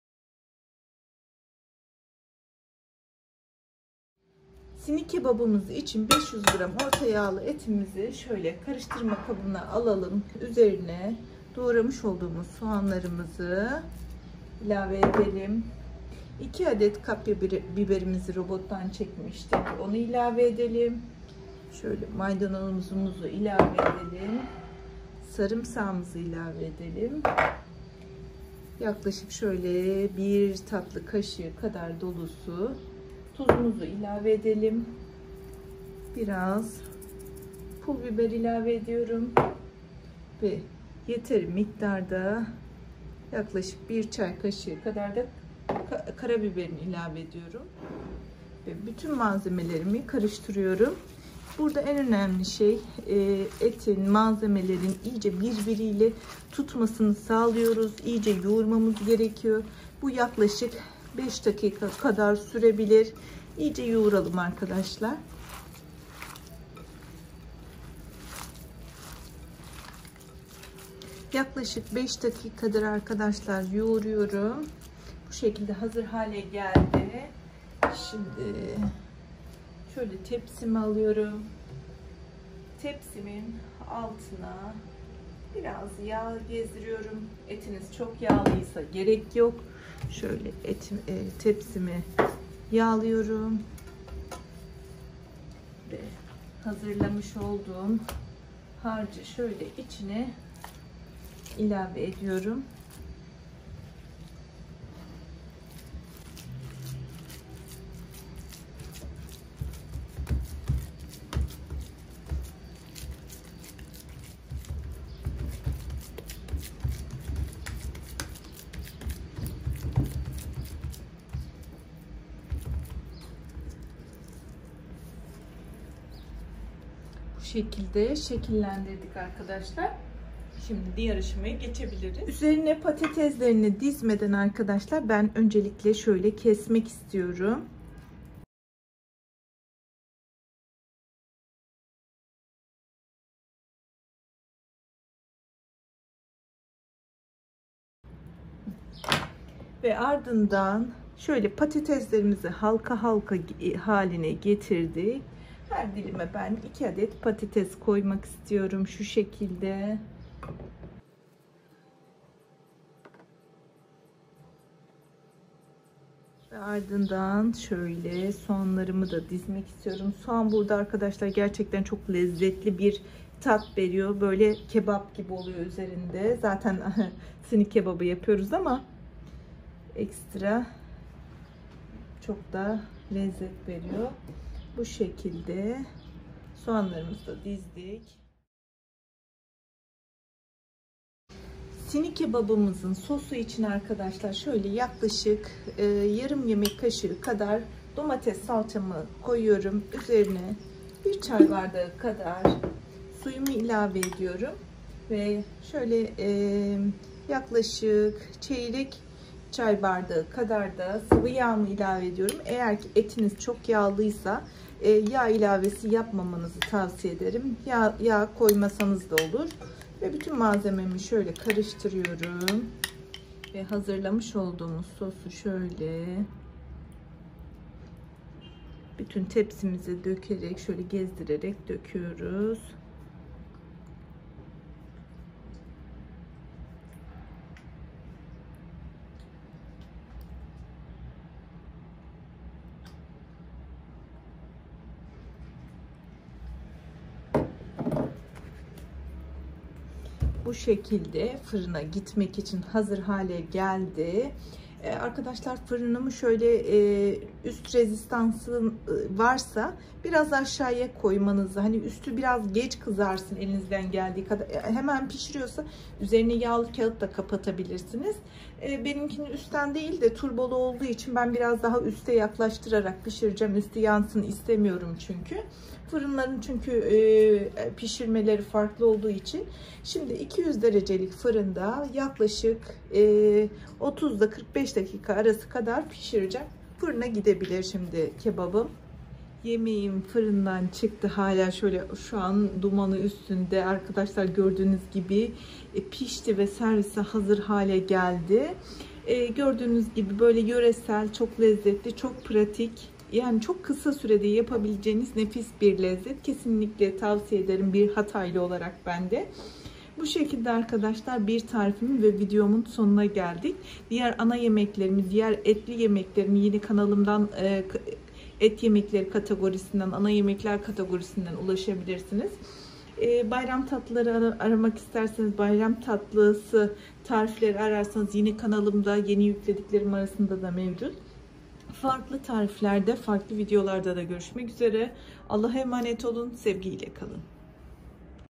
SİNİ KEBABINI BİRDE BENİM TARİFLE DENEYİN. 👌🏻😋 HATAYLILAR BU İŞİ BİLİYOR ✔️💯
Malzemeler
500 Kıyma
2 adet Soğan
4-5 diş sarımsak
Yarım Demet Maydanoz
2 kapya biber
Tuz
Pulbiber
Kimyon
Karabiber

Üzerine ;
Patates
Soğan

Sosu için;
1 tatlı kaşığı domates salçası
1 çay bardağı su
Sıvıyağ